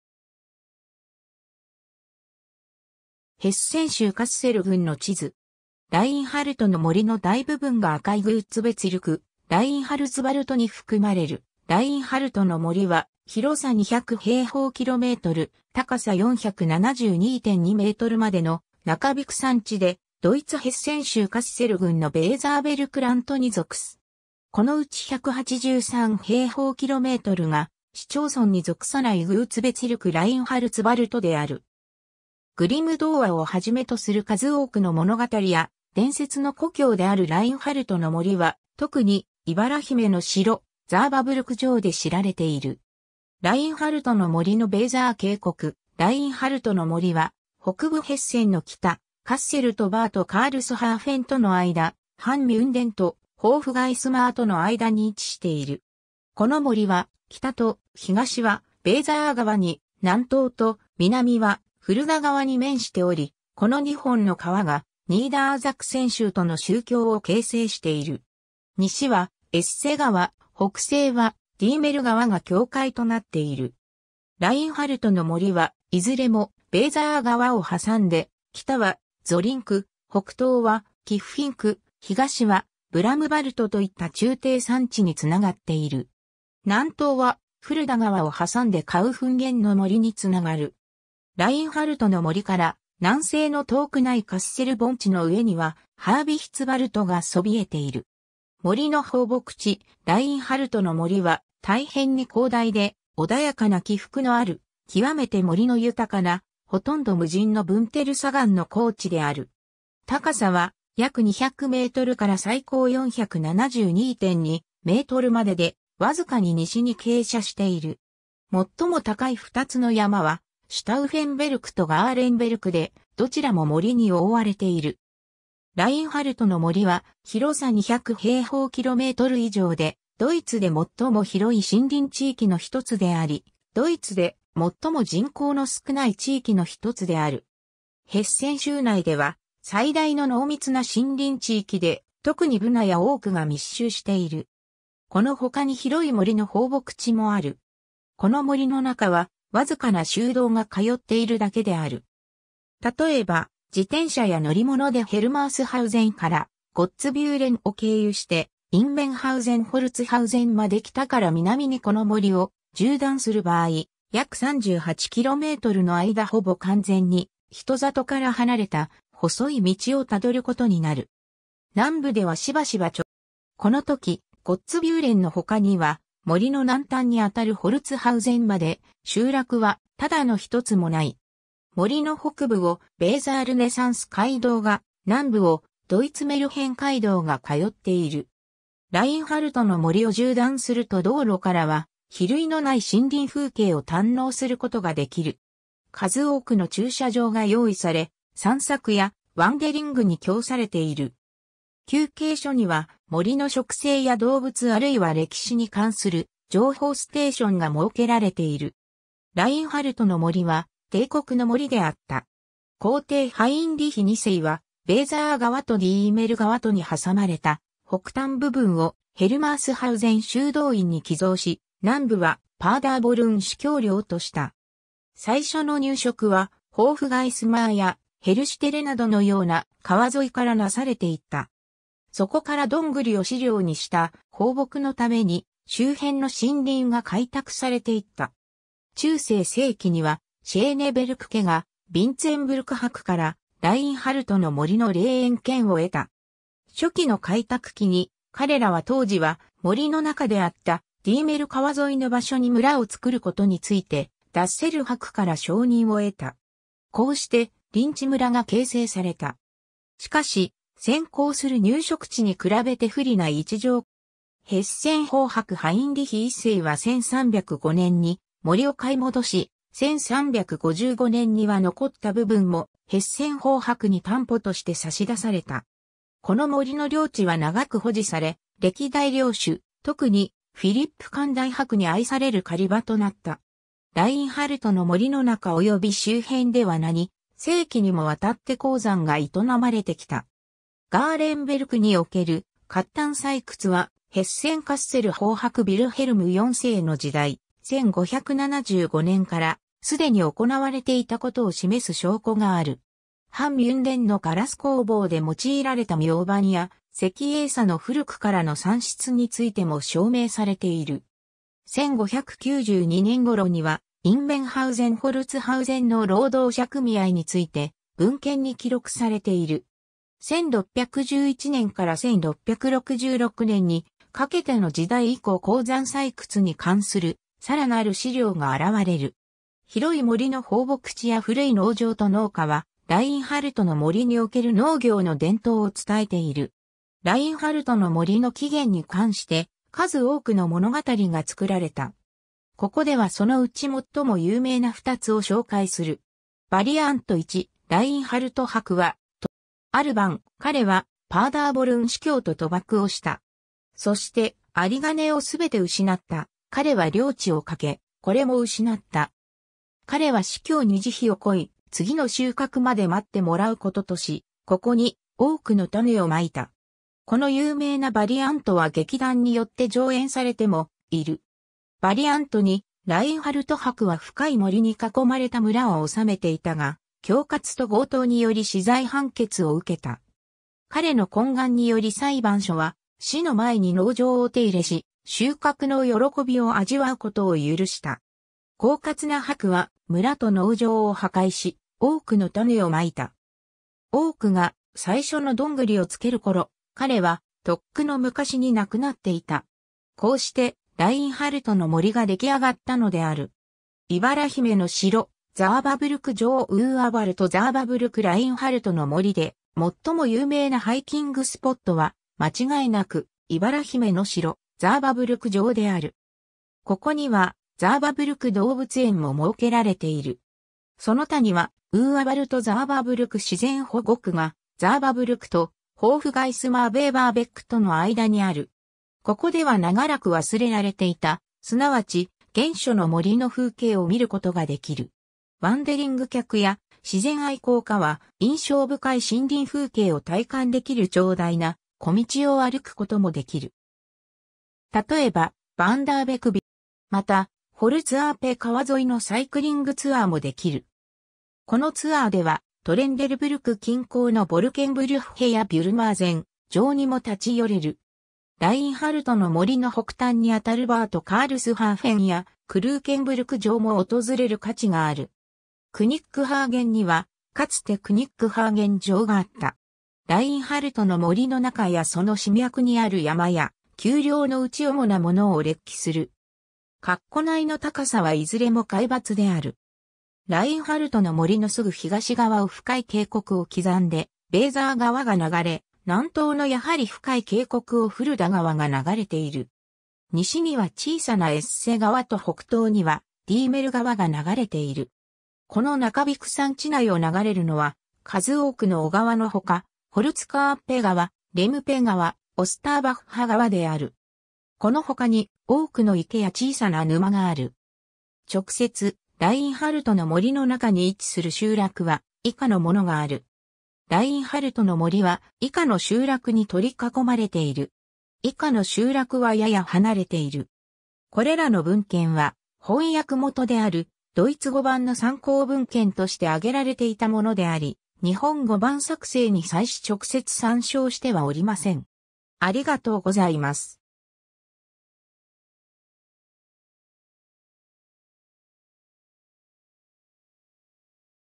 ラインハルトの森
ドイツ・メルヘン街道
ヘッセン州カッセル郡の地図。
ラインハルトの森の大部分が赤いグーツベツィルク・ラインハルツヴァルトに含まれる ラインハルトの森 (Reinhardswald) は広さ 200 km2、高さ 472.2 m までの中低山地で、ドイツヘッセン州カッセル郡のヴェーザーベルクラントに属す。
このうち 183 km2が、市町村に属さないグーツベツィルク・ラインハルツヴァルトである。
 グリム童話をはじめとする数多くの物語や伝説の故郷であるラインハルトの森は、特に『いばら姫（眠れる森の美女）』の城、ザーバブルク城で知られている。
 ラインハルトの森のヴェーザー渓谷 ラインハルトの森は、北部ヘッセンの北、カッセルとバート・カールスハーフェンとの間、ハン・ミュンデンとホーフガイスマーとの間に位置している。
この森は、北と東はヴェーザー川に、南東と南はフルダ川に面しており、この2本の川がニーダーザクセン州との州境を形成している。
西はエッセ川、北西はディーメル川が境界となっている。
ラインハルトの森は、いずれもヴェーザー川をはさんで、北はゾリンク、北東はキッフィンク、東はブラムヴァルトといった中低山地につながっている。
南東はフルダ川をはさんでカウフンゲンの森につながる。
ラインハルトの森から南西の遠くないカッセル盆地の上にはハービヒツヴァルトがそびえている。
 森の放牧地 ラインハルトの森は、大変に広大で、穏やかな起伏のある、極めて森の豊かな、ほとんど無人のブンテル砂岩の高地である。
高さは約 200 m から最高 472.2 m までで、わずかに西に傾斜している。
最も高い2つの山は、シュタウフェンベルク (472.2 m) とガーレンベルク (472.1 m) で、どちらも森に覆われている。
 ラインハルトの森は、広さ200 km2以上で、ドイツで最も広い森林地域の1つであり、ドイツで最も人口の少ない地域の1つである。
ヘッセン州内では最大の濃密な森林地域で、特にブナやオークが密集している。
この他に広い森の放牧地もある。
 この森の中はわずかな州道が通っているだけである。
たとえば、自転車や乗り物でヘルマースハウゼン（バート・カールスハーフェン）からゴッツビューレンを経由してインメンハウゼン＝ホルツハウゼンまで北から南にこの森を縦断する場合、約 38 km の間ほぼ完全に人里から離れた細い道をたどることになる。
南部ではしばしば直線のコースが現れる。
この時、ゴッツビューレンの他には森の南端に当たるホルツハウゼンまで、集落はただの一つもない。
森の北部をヴェーザールネサンス街道が、南部をドイツ・メルヘン街道（『いばら姫』ルート）が通っている。
 ラインハルトの森を縦断すると道路からは比類のない森林風景を堪能することができる。
数多くの駐車場が用意され、散策やワンデリングに供されている。
休憩所には森の植生や動物あるいは歴史に関する情報ステーションが設けられている。
 ラインハルトの森は帝国の森であった。
皇帝ハインリヒ2世は、ヴェーザー川とディーメル川とにはさまれた北端部分をヘルマースハウゼン修道院に寄贈し、南部はパーダーボルン司教領とした。
最初の入植は、ホーフガイスマーやヘルシュテレなどのような、川沿いからなされていった。
そこからドングリを飼料にした放牧のために周辺の森林が開拓されていった。
中世盛期にはシェーネベルク家がヴィンツェンブルク伯からラインハルトの森のレーエン権を得た。
初期の開拓期に彼らは当時は森の中であったディーメル川沿いの場所に村を造ることについてダッセル伯から承認を得た。
こうして林地村が形成された。
しかし、先行する入植地に比べて不利な位置条件 や創設した領主の衰退などによりこれらは廃村となっていった。
ヘッセン方伯ハインリヒ1世は1305年に森を買い戻し、1355年には残った部分もヘッセン方伯に担保として差し出された。
この森の領地は永く保持され、歴代領主、特にフィリップ寛大伯に愛される狩場となった。
 ラインハルトの森の中および周辺では何世紀にもわたって鉱山が営まれてきた。
ガーレンベルクにおける褐炭採掘（1842年から1970年まで採掘がなされていた）は、ヘッセン＝カッセル方伯ヴィルヘルム4世の時代、1575年から既に行われていたことを示す証拠がある。
ハン・ミュンデンのガラス工房で用いられたミョウバンや石英砂の古くからの産出についても証明されている。
1592...